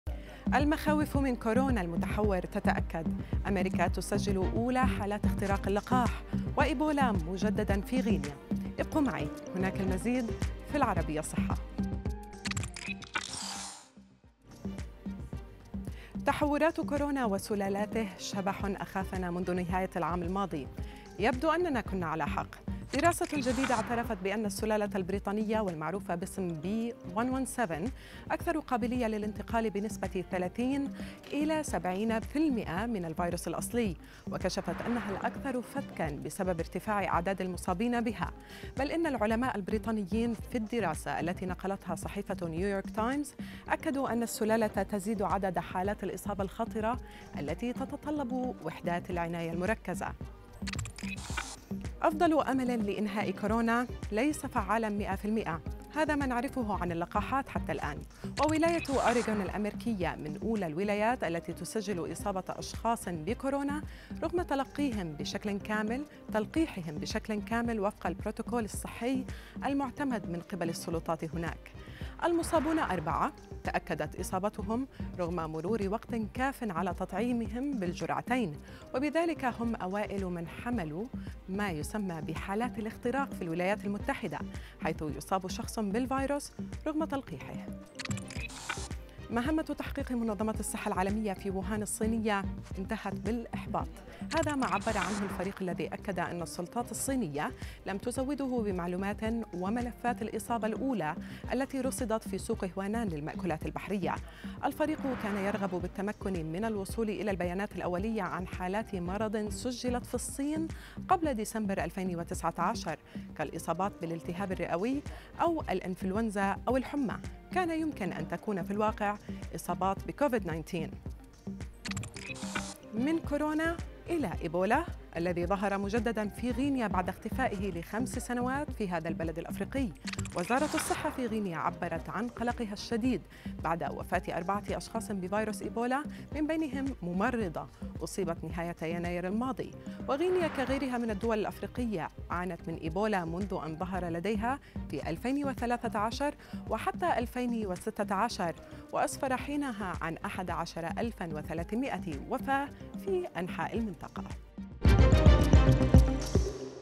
أخبار الصحة في دقيقتين
🦠 المخاوف من كورونا "المتحور" تتأكد
‏🇺🇸 أميركا تسجل أولى حالات اختراق اللقاح
‏🇬🇳 وإيبولا مجددا في غينيا